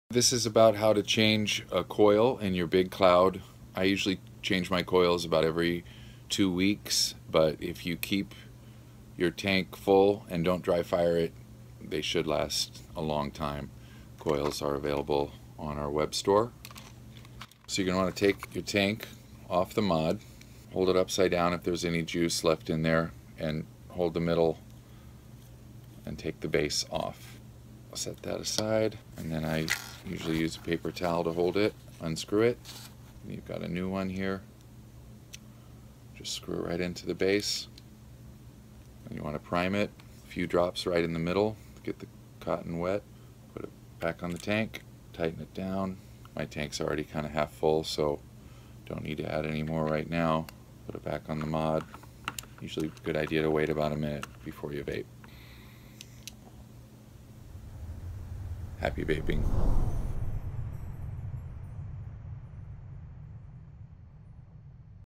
311's Grassroots Big Cloud coil changing tutorial
Learn how to change the coils in your Grassroots Big Cloud.